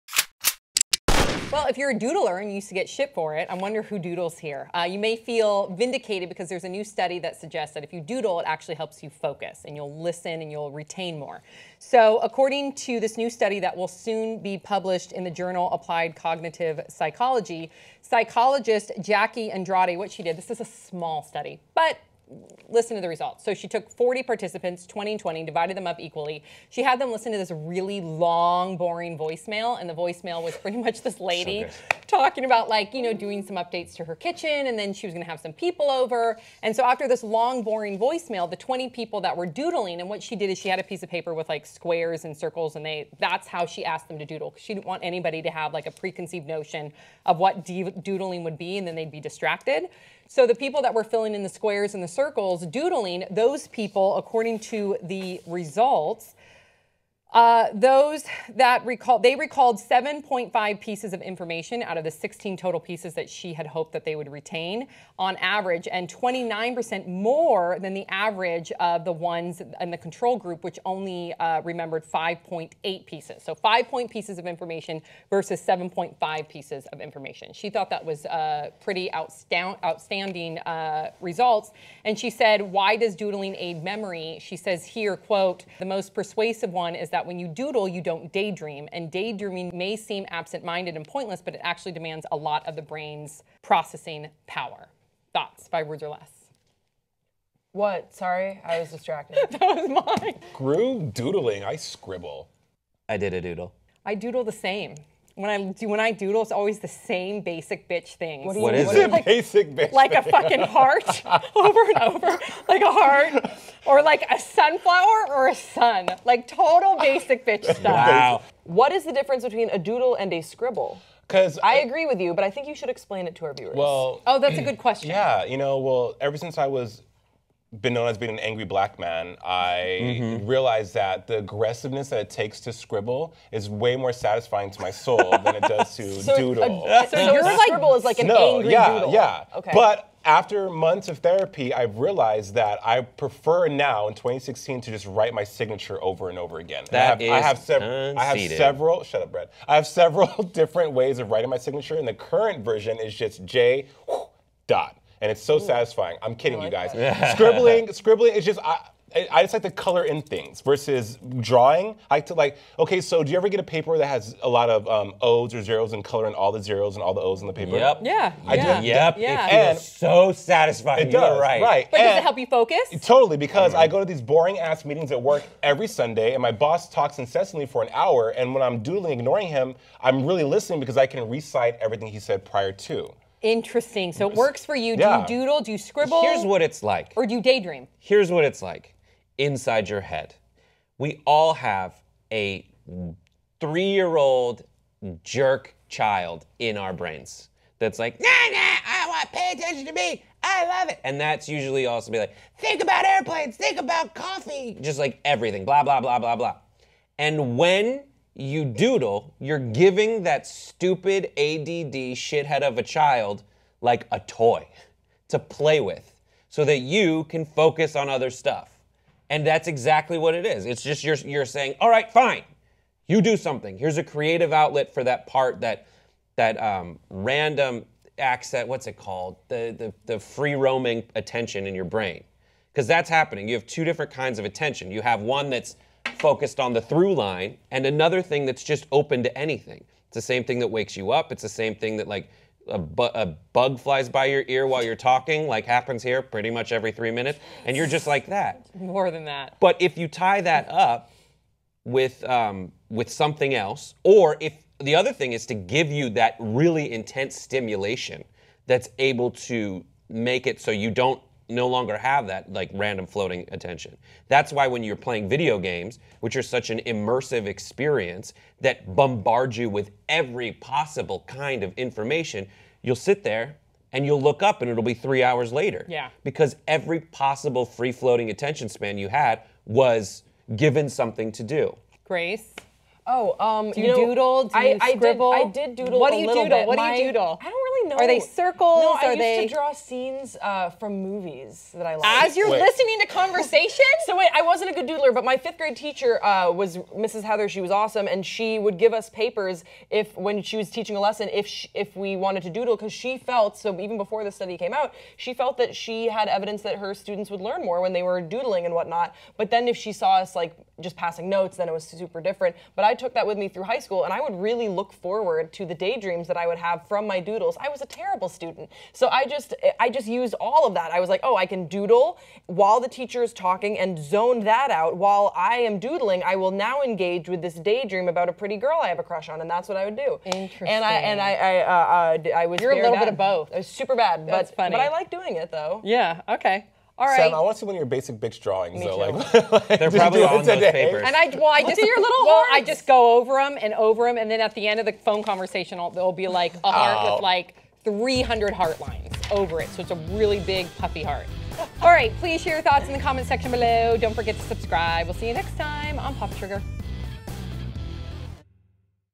Doodling Helps You Pay Attention
If you're a doodler you may feel vindicated about all the crap you got from your teachers. Sam, Grace, Brett, and Jason Carter discuss how doodling helps you pay attention. Let us know if you doodle.